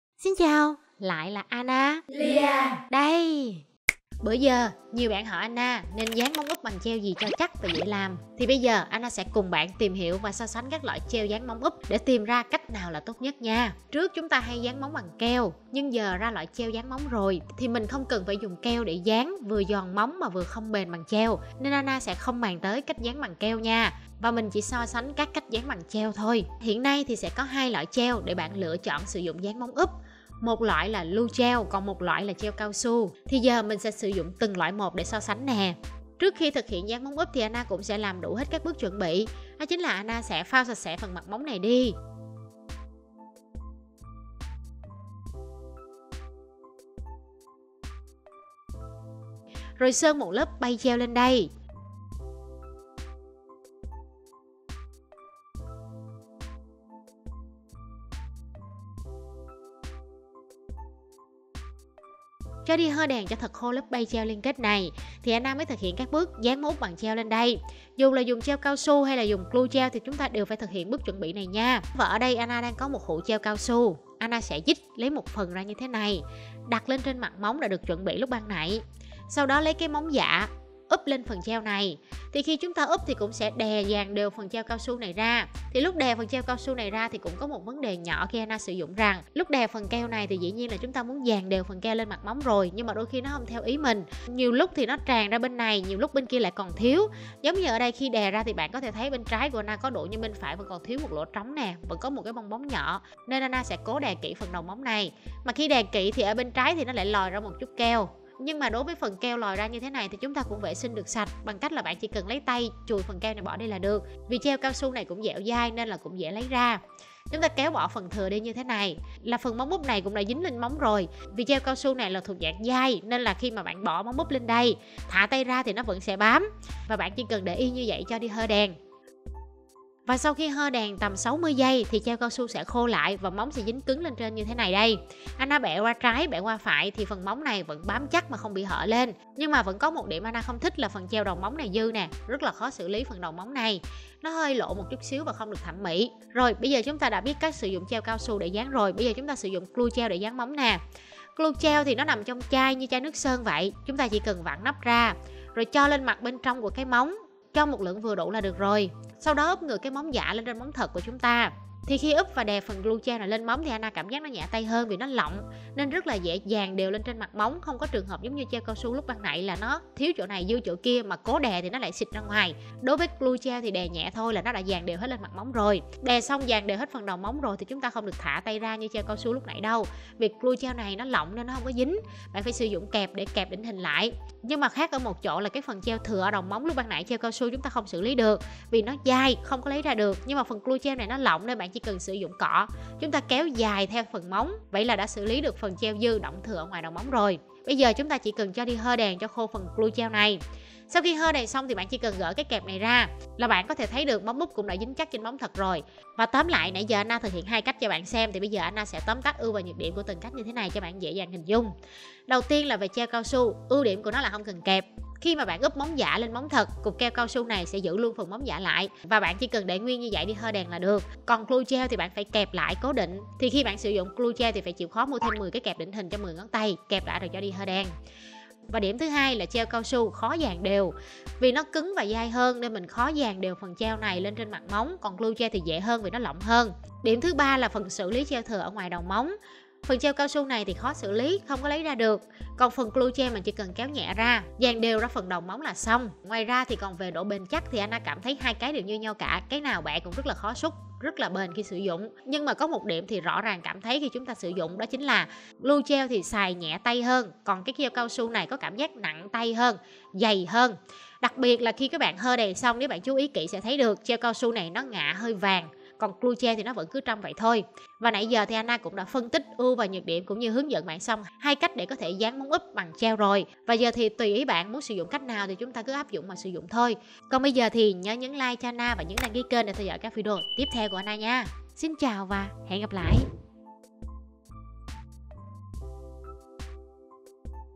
Tìm Hiểu 2 Cách Dán Móng Tay Giả Phổ Biến - ANALIA Nails
Dán móng tay giả thì có nhiều cách lắm nha mọi người, nhưng nay ANA lọc ra 2 cách dán móng thông dụng nhất để mọi người cùng tham khảo đây ạ.